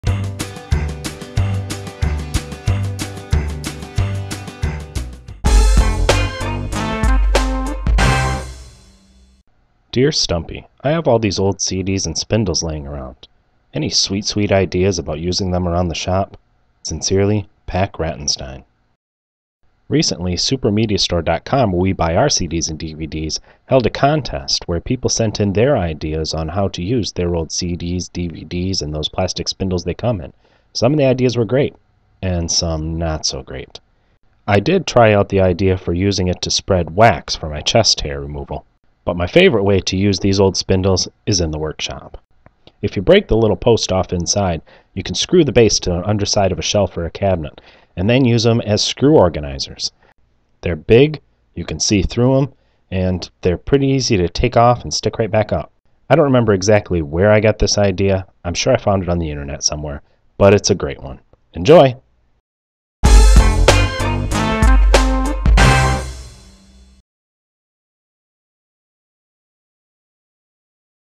Use Old CD Spindles for Fastener Storage -Woodworking With Stumpy Nubs: Stumpy Short #3
Stumpy shows a fan how to use old CD spindles for screw storage and chest hair removal.